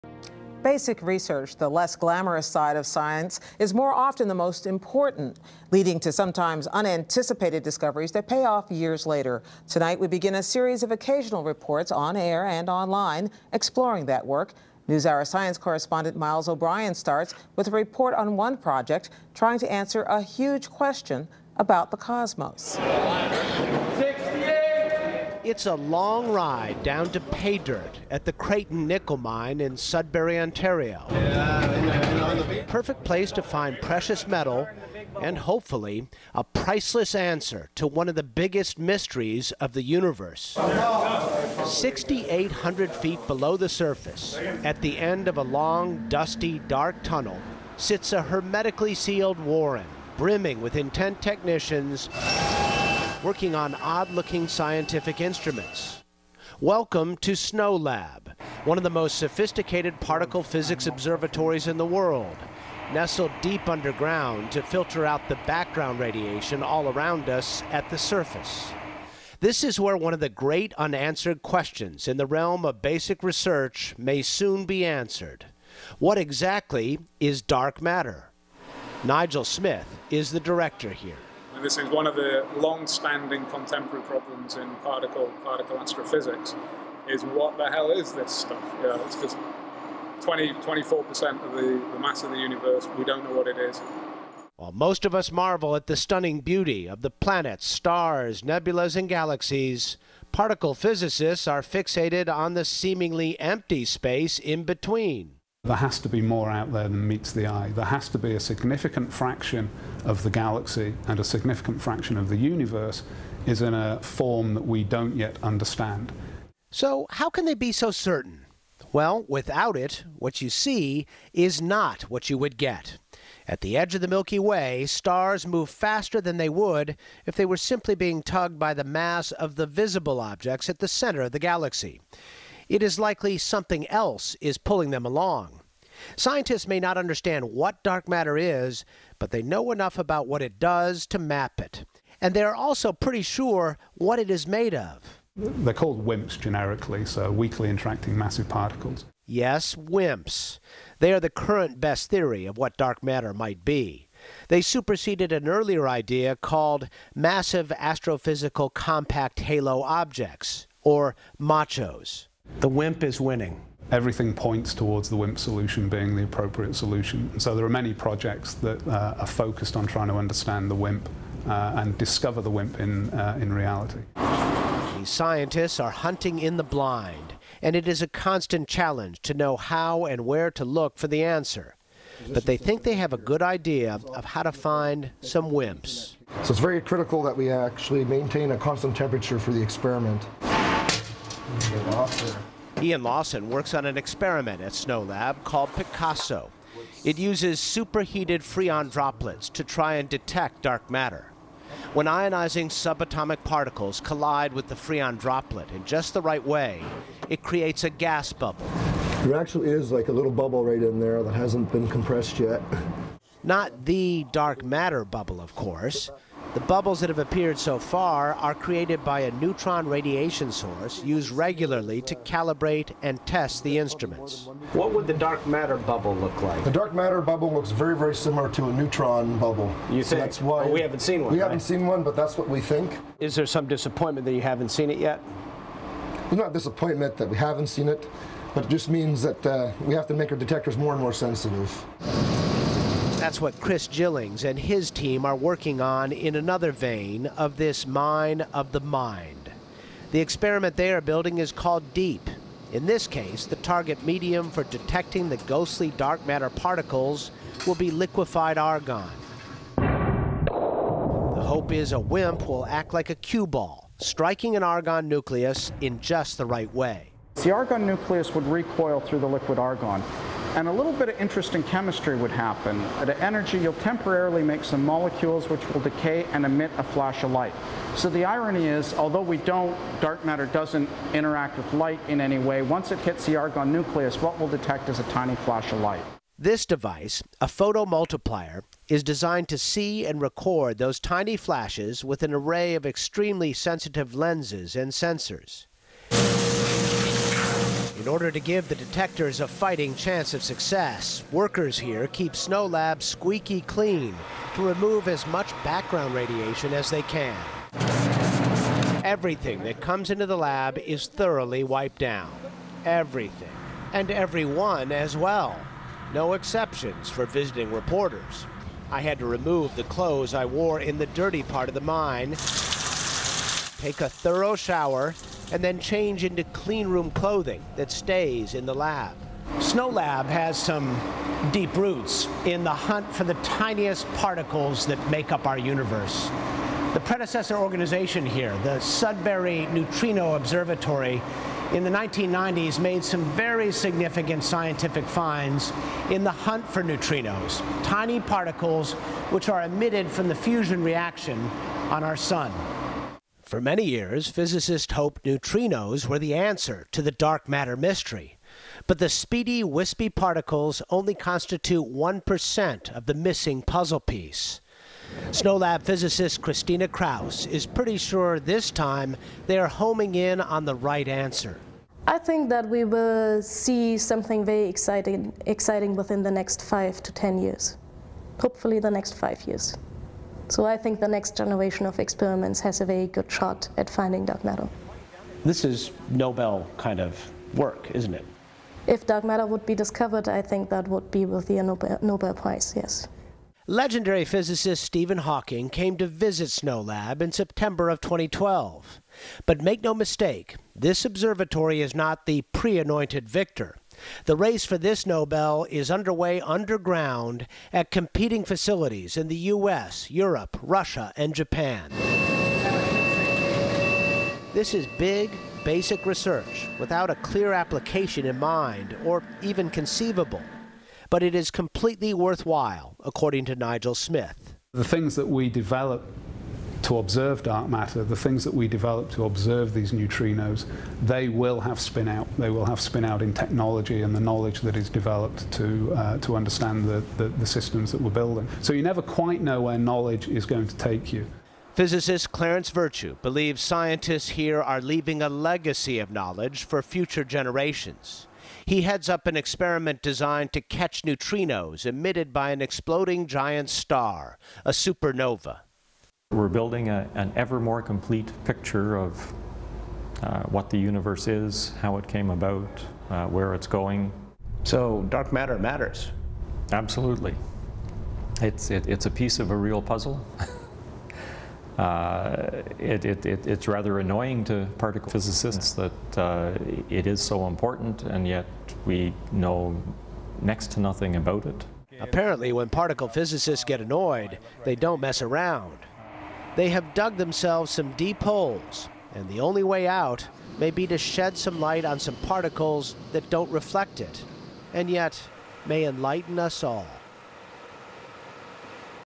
Hunt for Dark Matter 6,800 Feet Underground
At the bottom of a nickel mine near Sudbury, Ontario, scientists at one of the world's most sophisticated particle physics observatories are investigating one of the biggest mysteries of the cosmos: What is dark matter? Science correspondent Miles O'Brien helps to shed some light on the research at SNOLAB.